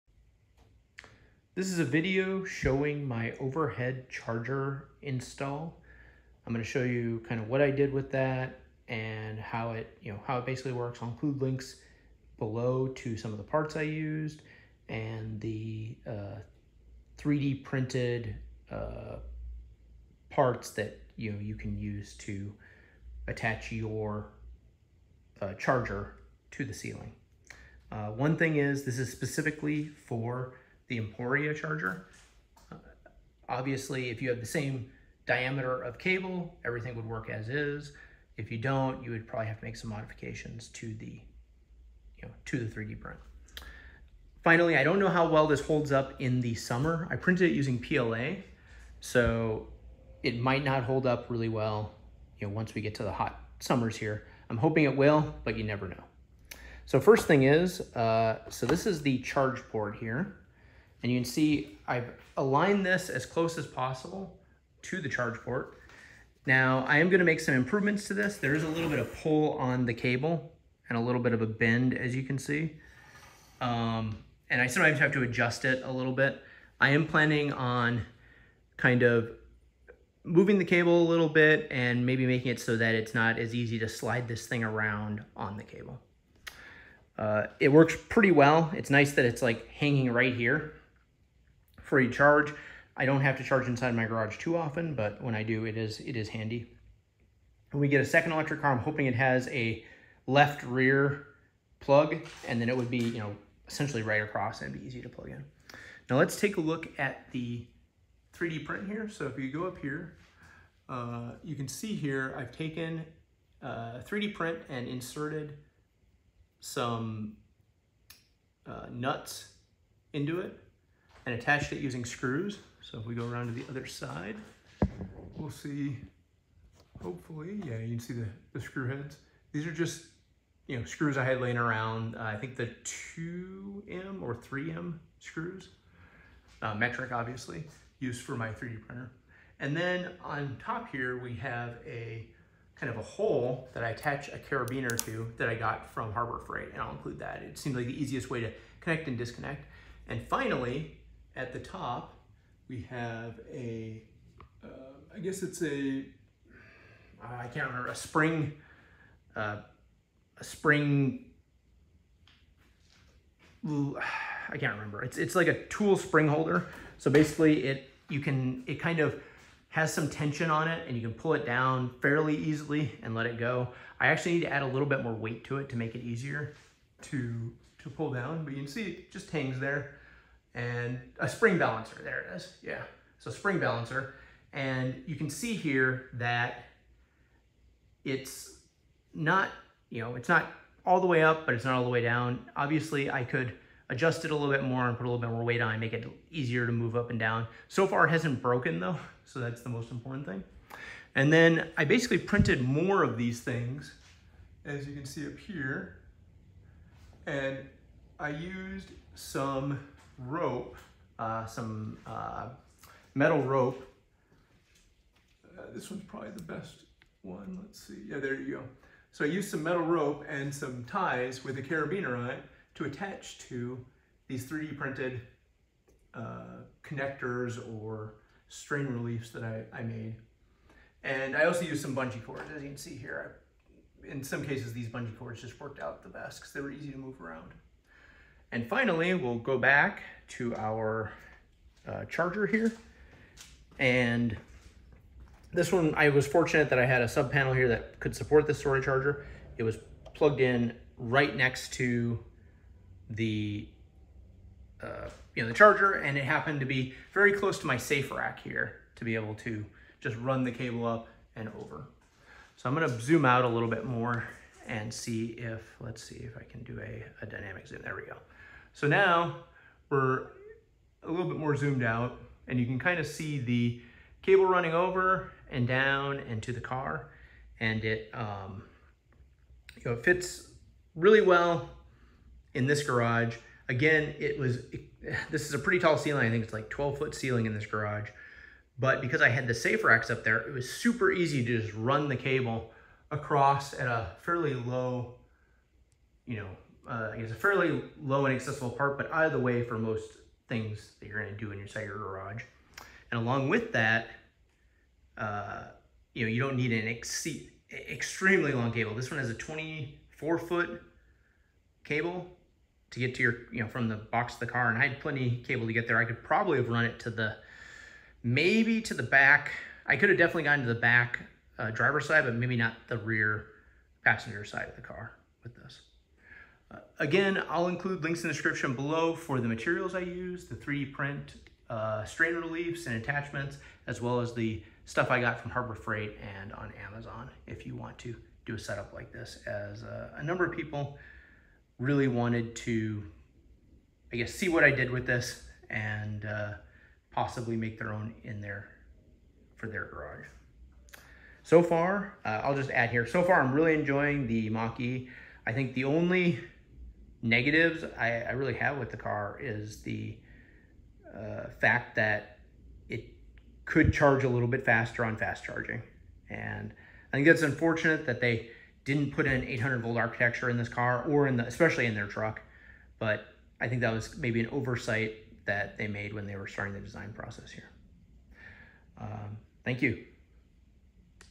Overhead EV charger - overhead evse emporia level 2 charger
This is my overhead EVSE (Emporia Level 2 EV Charger) Charging solution. Heres the things I used:

M2/M3 nuts/bolts